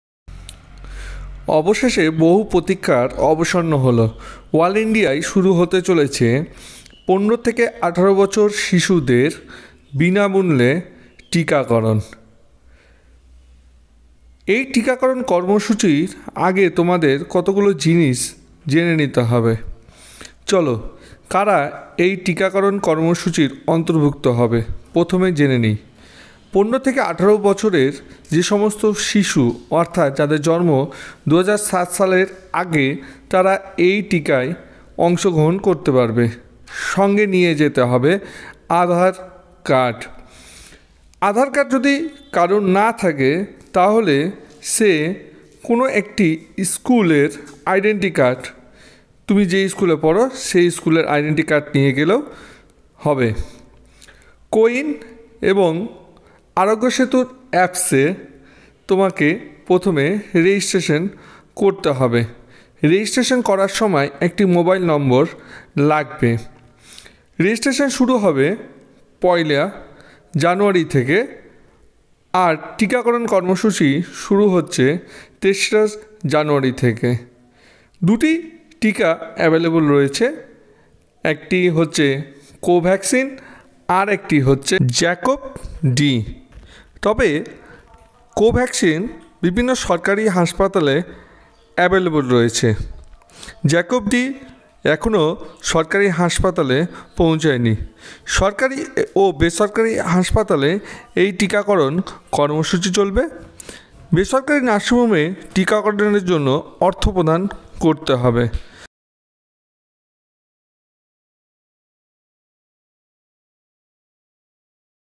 শিশুদের টিকাকরণ কর্মসূচি 2022//কারা পাবে কোথায় দেওয়া হবে কি ডকুমেন্টস লাগবে/india corona vaccine
অল ইন্ডিয়া  ভ্যাকসিন টিকাকরণ 2022